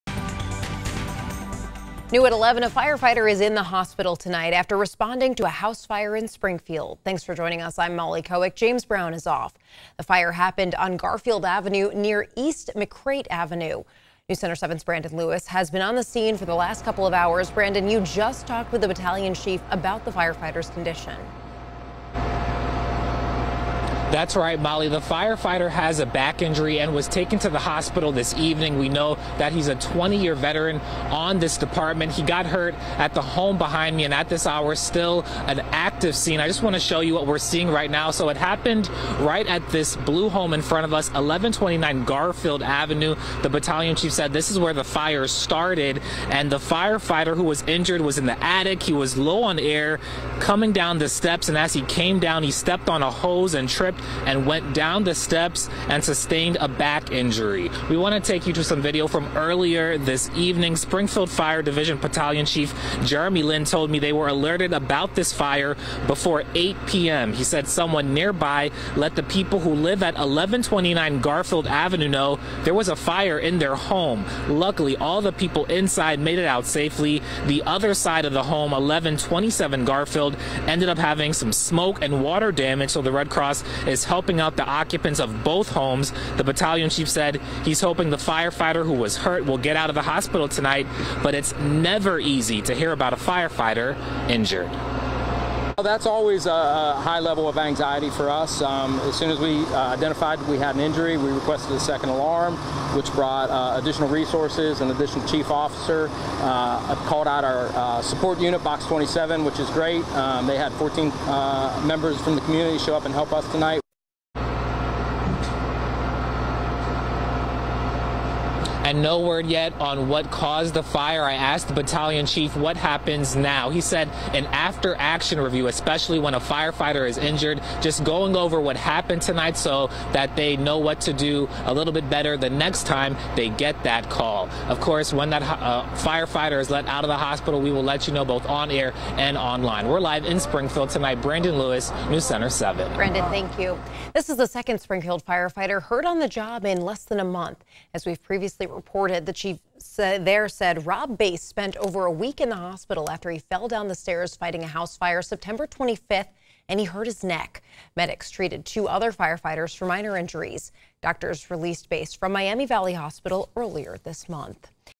Firefighter hurts back, taken to hospital after fire in Springfield | WHIO-TV
Crews were called to a fire in the 1000 block of Garfield Avenue around 7:40 p.m., according to Springfield Fire Division Battalion Chief Jeremy Linn. READ THE STORY